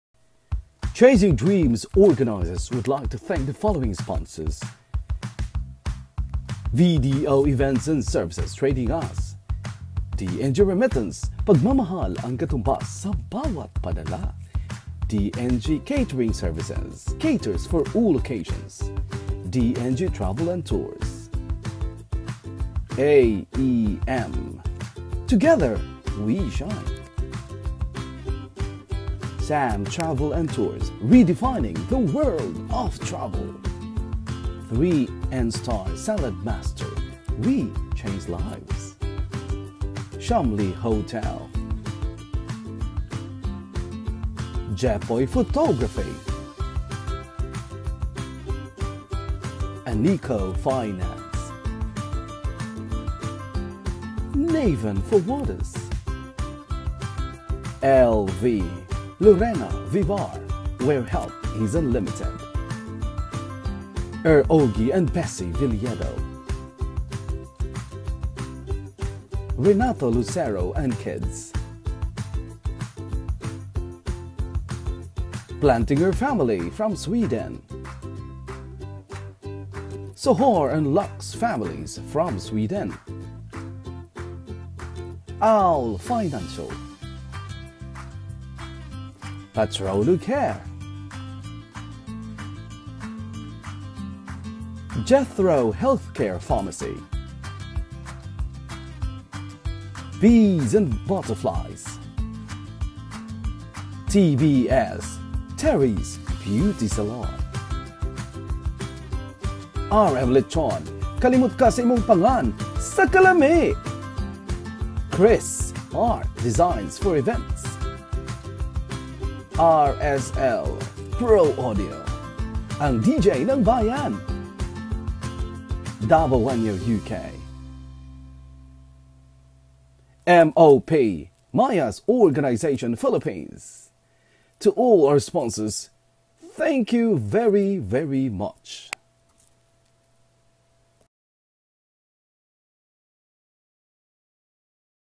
Chasing Dreams Sponsors
Chasing Dreams is a benefit concert organised to raise funds for my journey to the World Championships of Performing Arts (WCOPA) from 4th to 16th of July 2018 taking place in Long Beach, California.